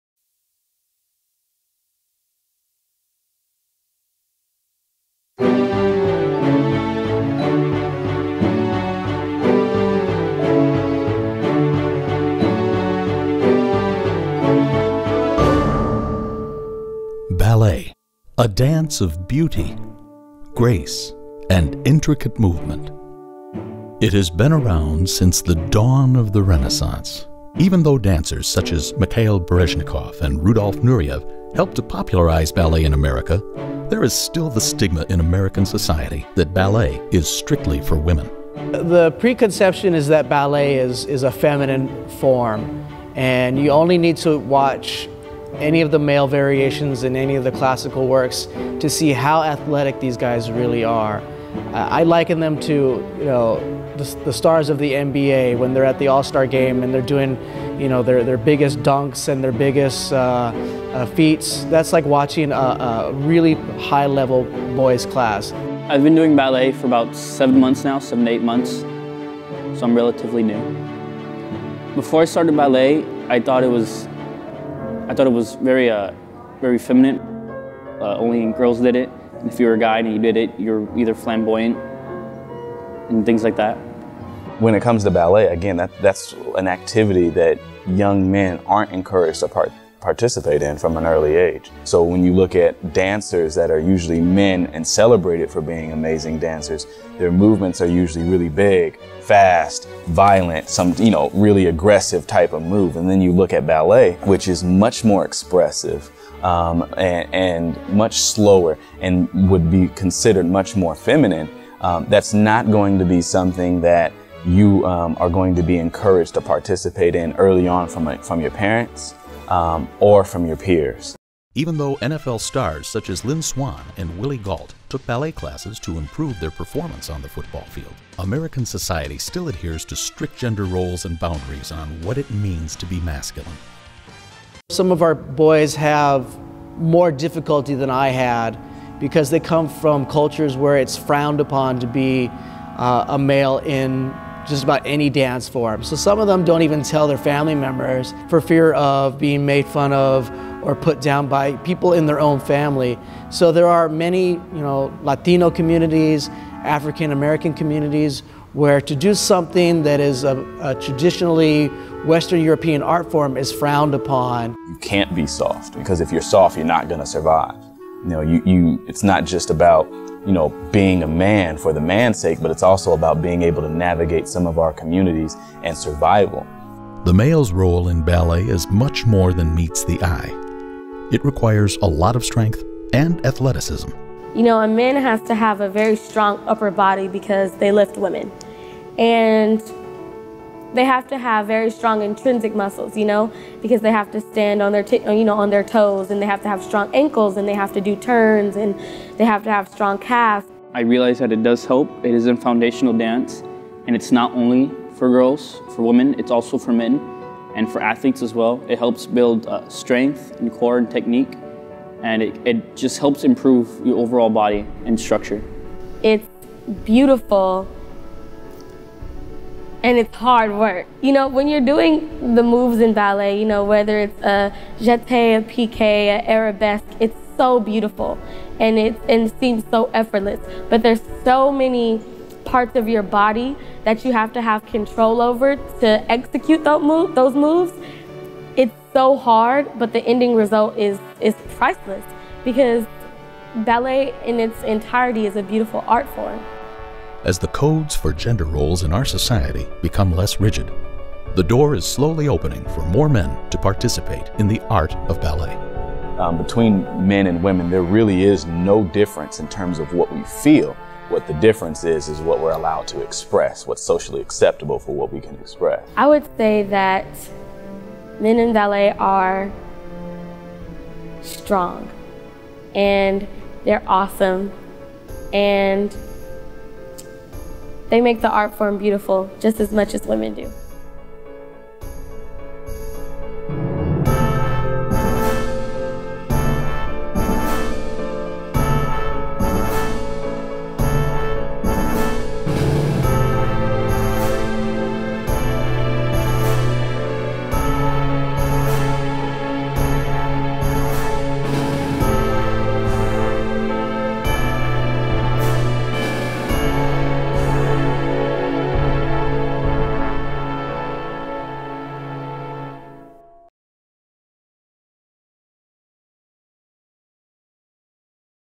Danseur
As a student project, my team and I authored mini-documentary discussing the role of men in ballet. Through ballet, we look at the stigmas men face in in western culture.